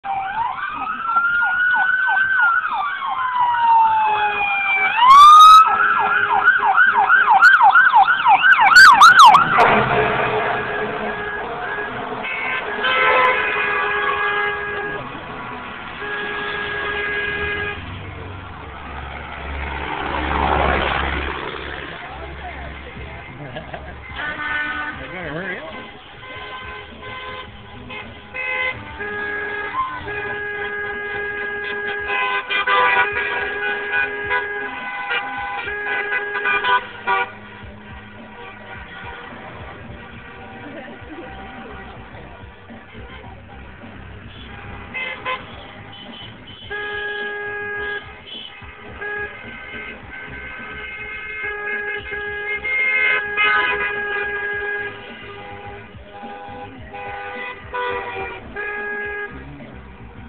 send off
Heuvelton lady bulldogs heading to the final four